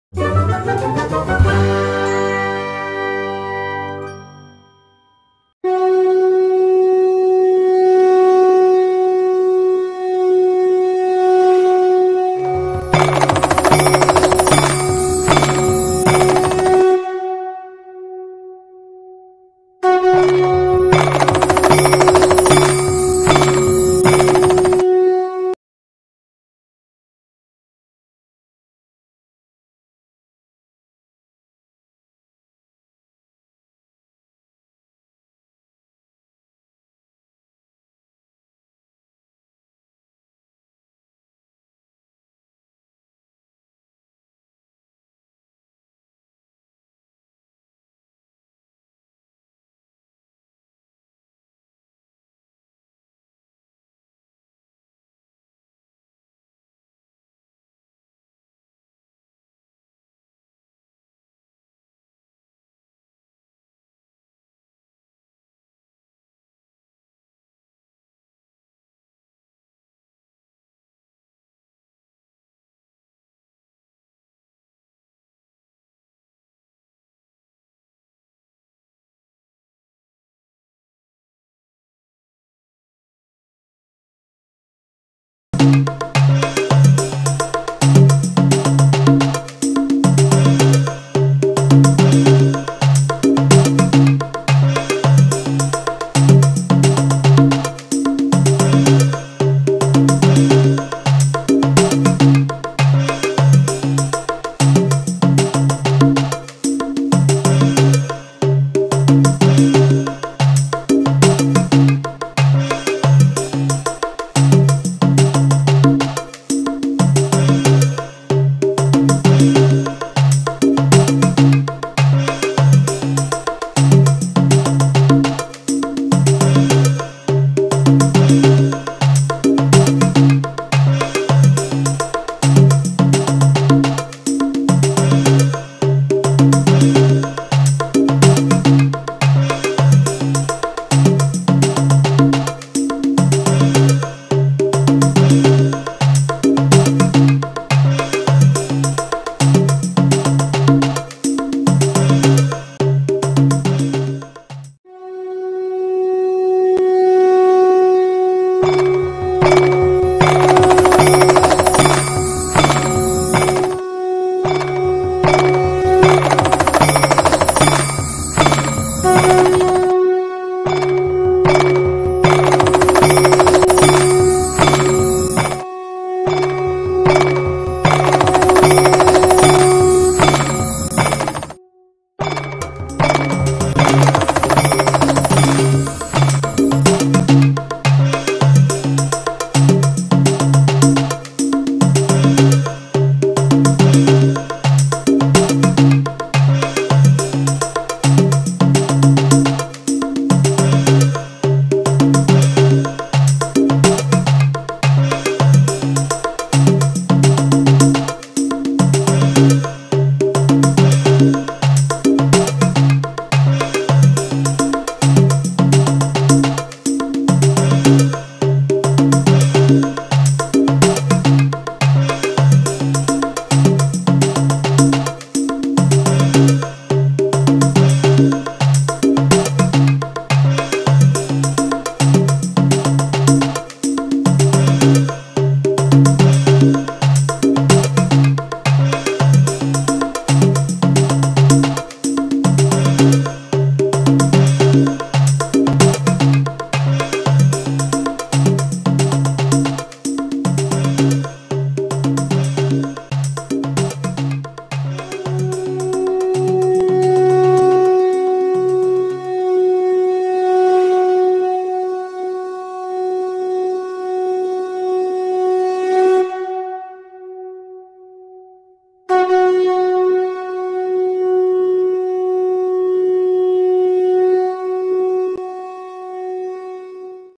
Colourpencilsketch Tandav AghoriPencil Sketch
The Aghori (Sanskrit aghora) are a small group of ascetic Shaiva sadhus. They engage in post-mortem rituals. They often dwell in charnel grounds, smear cremation ashes on their bodies, and use bones from human corpses for crafting kapalas (skull cups which Shiva and other Hindu deities are often iconically depicted holding or using) and jewellery. Their practices are sometimes considered contradictory to orthodox Hinduism

Many Aghori gurus command great reverence from rural populations, as they are supposed to possess healing powers gained through their intensely eremitic rites and practices of renunciation and tápasya.

Material used Bruno paper, 8b pencils, Steadler Mars Lumgraph 6b and 8B, Steadler Mars Lumgraph Black 48 2b
Sound Creation WalkBank app
Video Creation Powerdirector